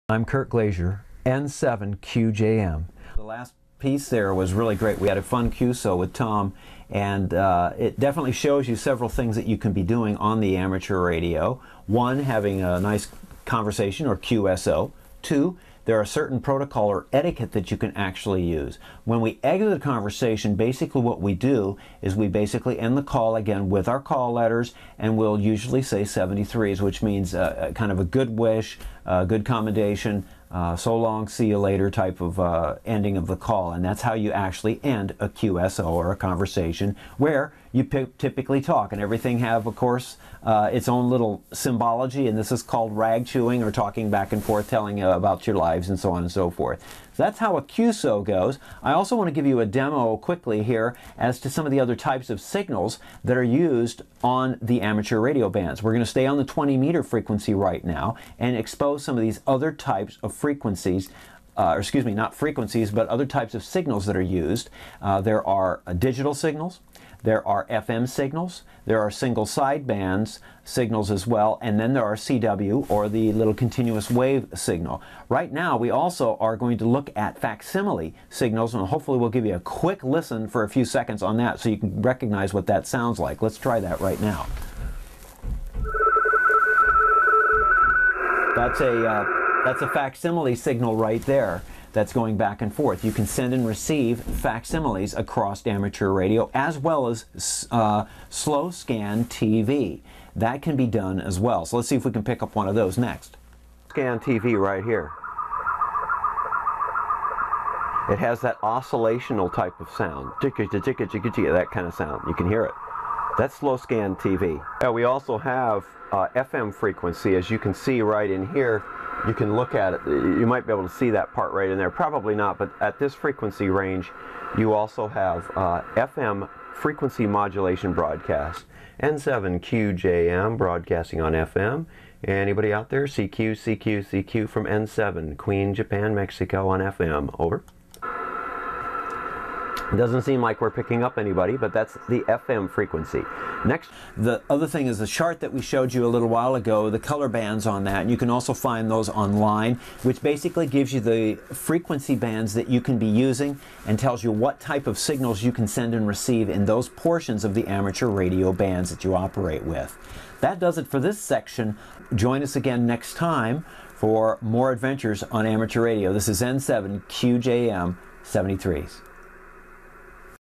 Types of Ham Radio Signals
Types of Ham Radio Signals. Part of the series: How to Use a Ham Radio. Learn detail on the many different types of ham radio signals in this free CB radio video.